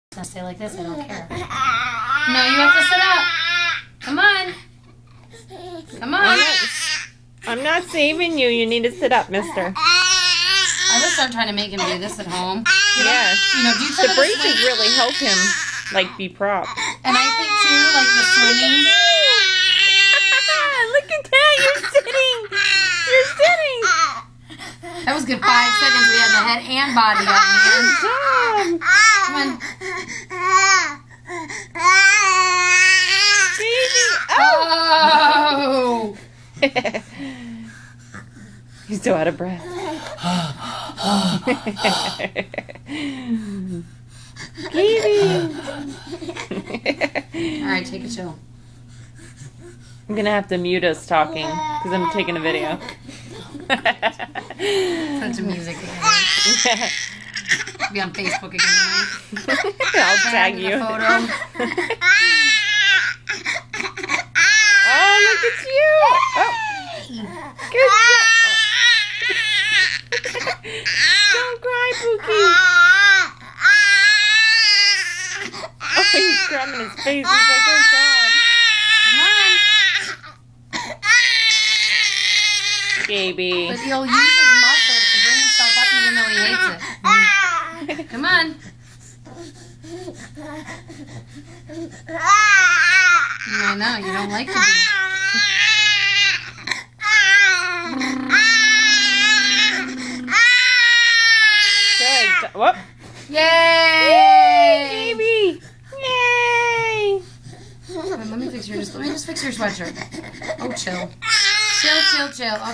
Gabriel sitting!
This is the second time Gabriel sat for a few secs!